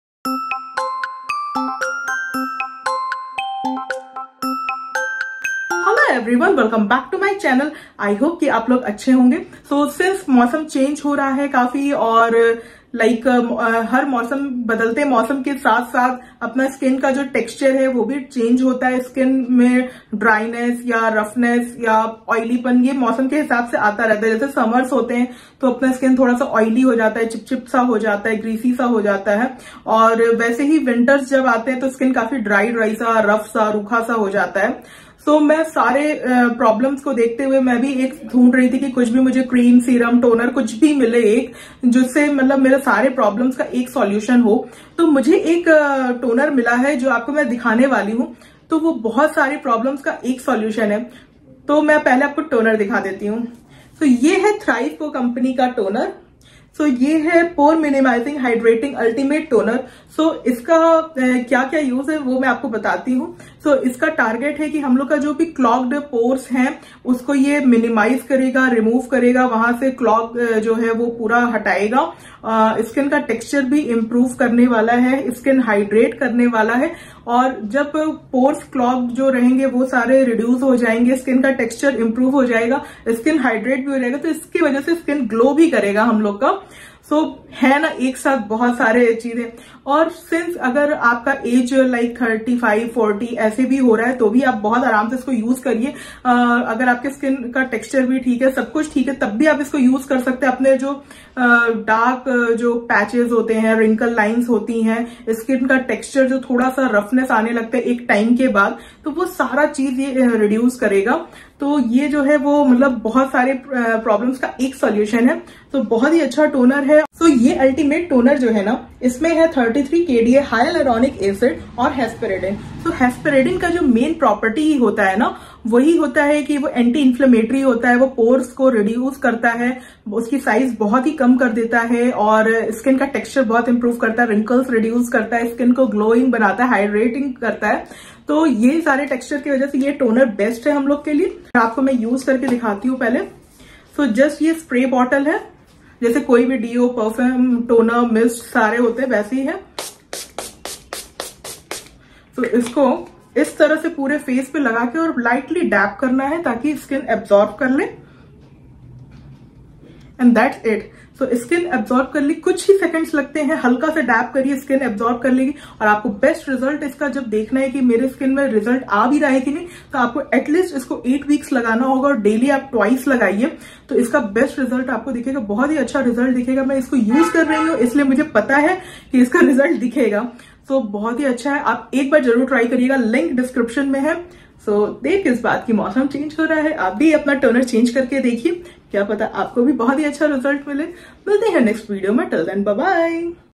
How to minimize pores? Here's a realistic solution from ThriveCo!
ThriveCo, a result oriented brand with science backed formulations have always impressed me with their product line. This time, it’s the Pore-Minimizing Ultimate Toner. Having had issues with enlarged pores as long as I can remember, I was really looking forward to using this product. And as expected, the product delivered what it promised. My pores have drastically reduced in size! My super sensitive skin didn’t react to any of the ingredients in the toner, instead the product was calming on my irritated skin. The combination of hesperidin and Hyaluronic Acid has impressively smoothened out my skin texture and evened out pigmentation too. Just look at the glow on my skin!